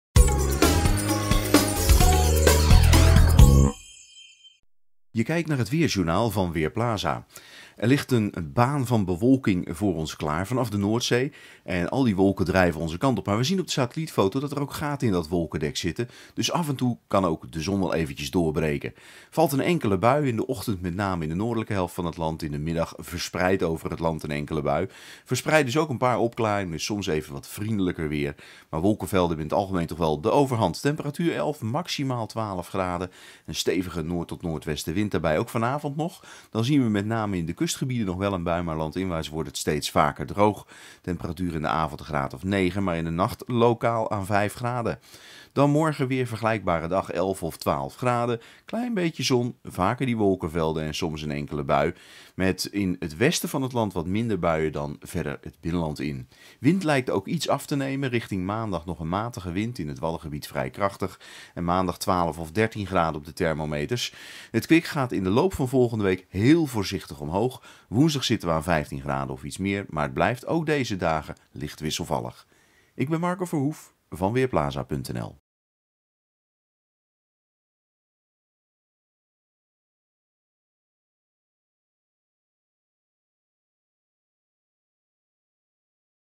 Weerbericht zaterdag: Vaak wolken, soms een bui
Er trekken wolken over het land, soms breekt even de zon door en af en toe valt er een bui. Verder is het fris, met maxima van slechts een graad of 11. Er waait een matige tot krachtige noordwestenwind. Ook de pinksterdagen zien er fris en wisselvallig uit. De temperatuur komt landinwaarts nauwelijks boven de 13 graden, aan zee en op de Wadden is het hooguit 10 of 11 graden. Er vallen wat buien maar soms breekt ook de zon even door. Na Pinksteren gaat de temperatuur iets omhoog maar het blijft wisselvallig.